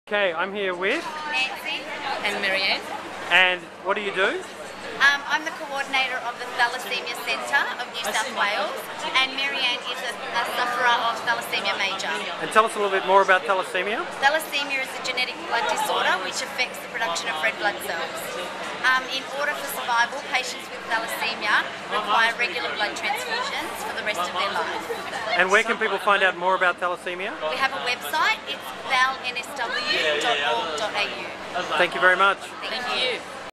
REELPEOPLE #23 - Nancy Lucich & Maryanne - Thalassaemia Society NSW
Meet Nancy Lucich and her Maryanne from New South Wales Thalassaemia Society in Sydney, Australia. This video taken in Sydney in April 2011 as part of Iggy Pintado's Reel People Project. The project aims to introduce 'real connections' via short video.